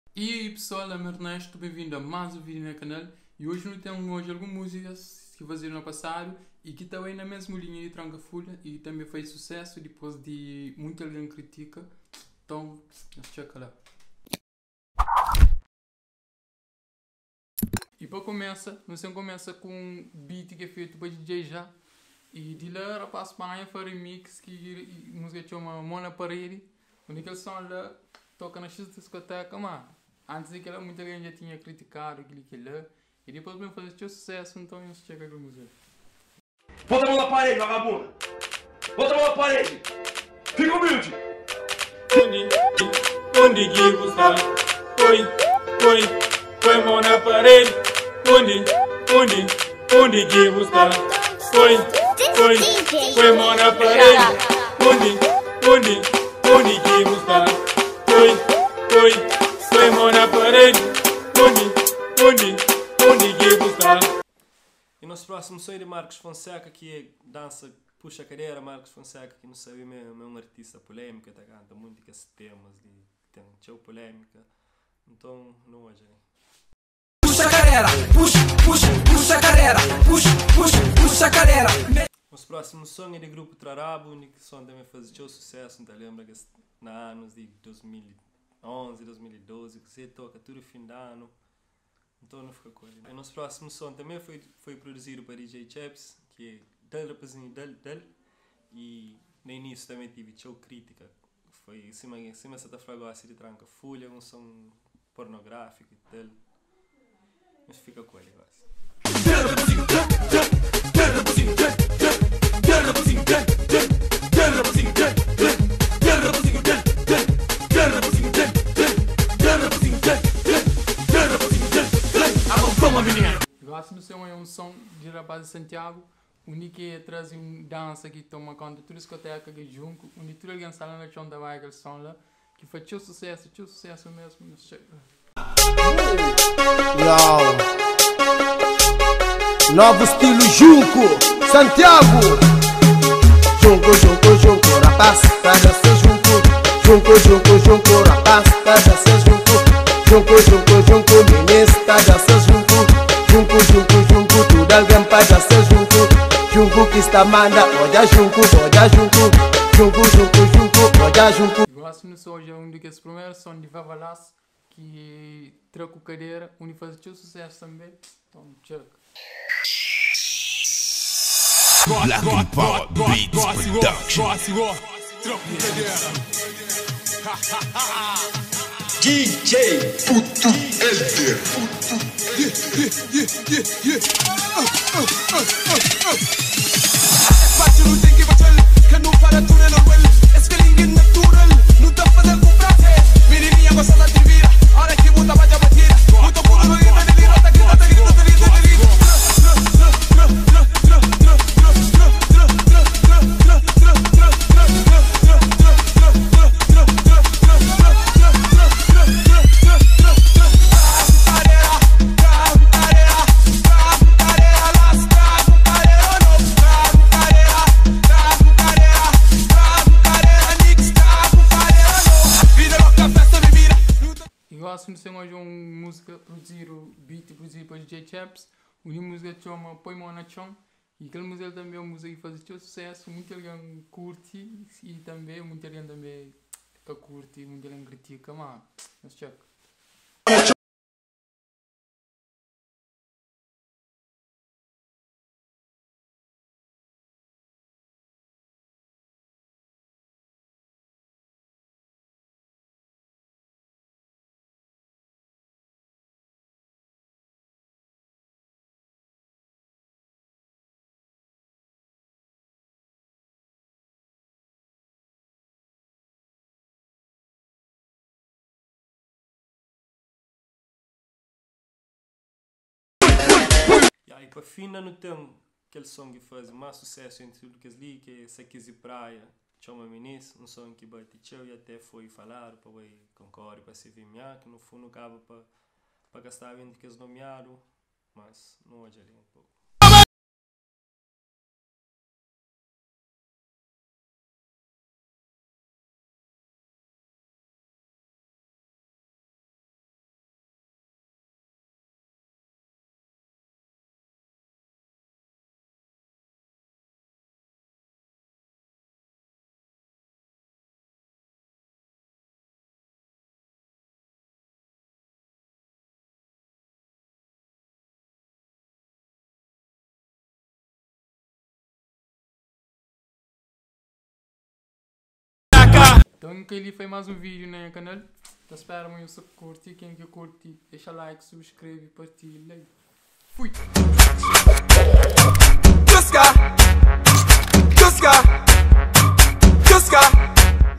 Mc Tranka fulha novo geração de batida!
Criar ou mudar para uma Página de criador de vídeos de jogos

Boas pessoal, keli é mais um vídeo na nha canal, e hoje nu ta bem lembra tempo ku musicas que fazi sucesso na passado sima Mc tranka fulha sta fazi...!!
nta spera ma nhos ta gosta e nhos subscreve!!